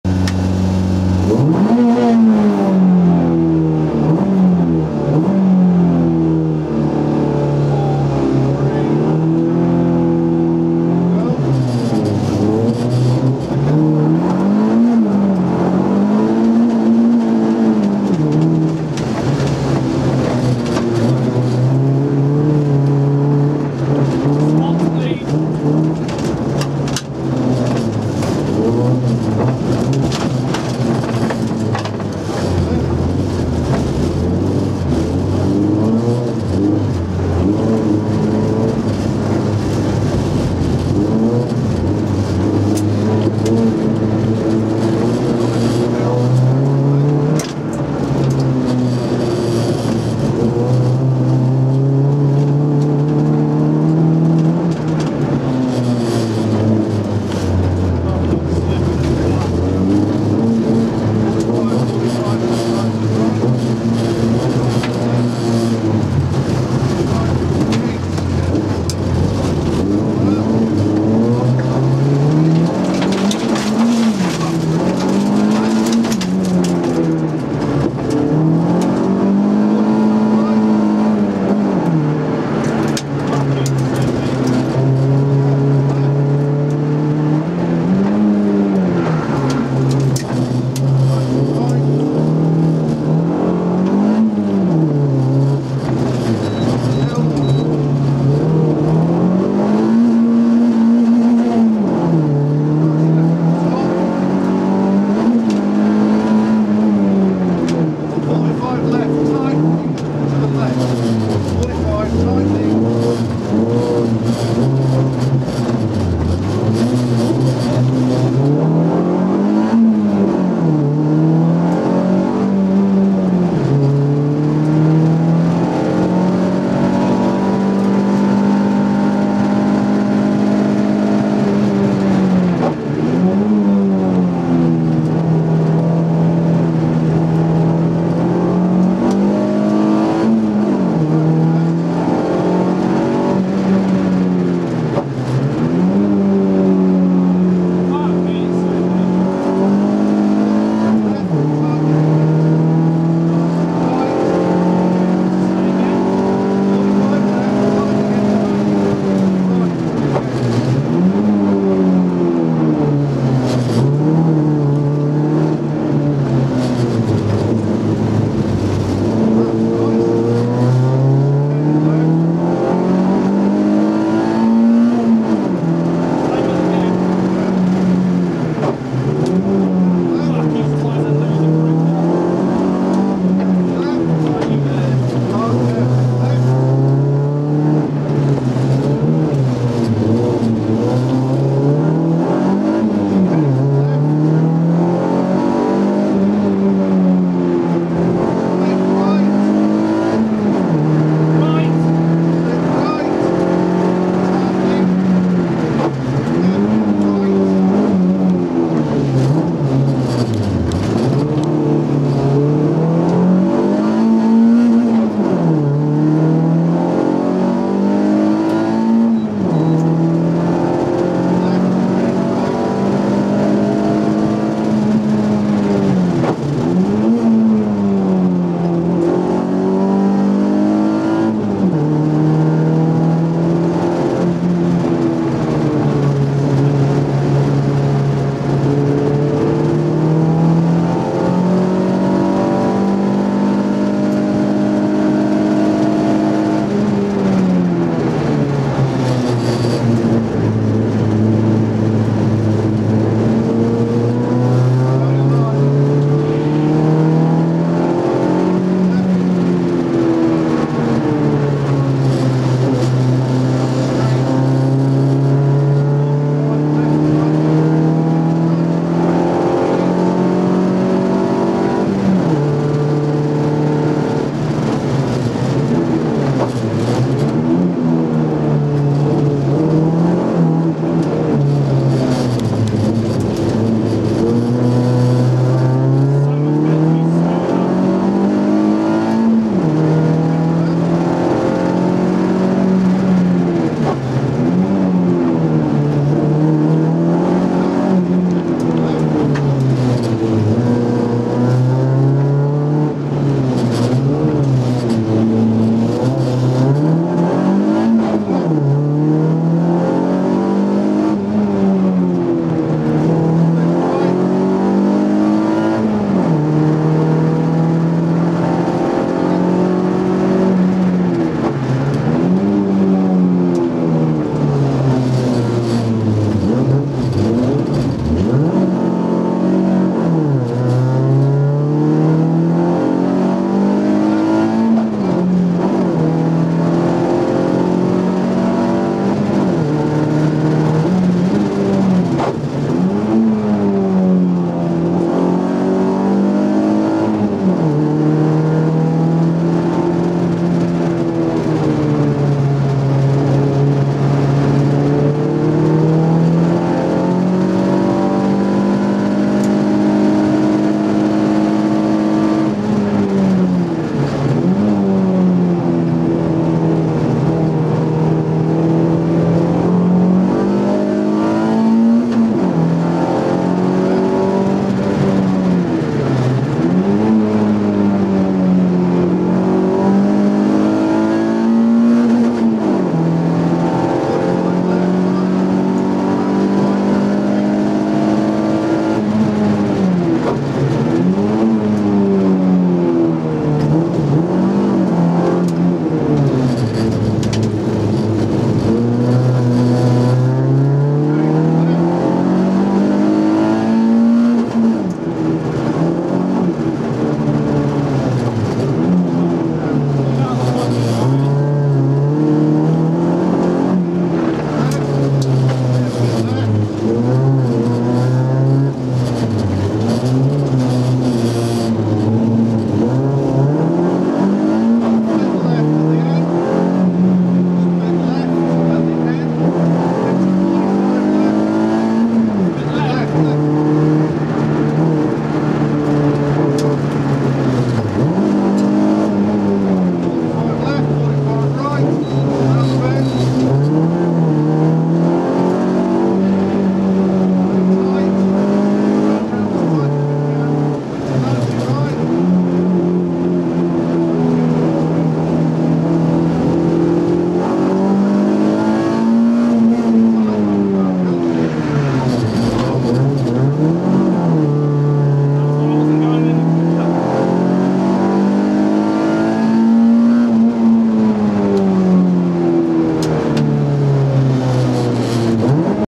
Brands Hatch Stages 2014 SS 7
MGJ Engineering Brands Hatch Stages 2014
Oliver & Simon Howard
Group A - Nissan Micra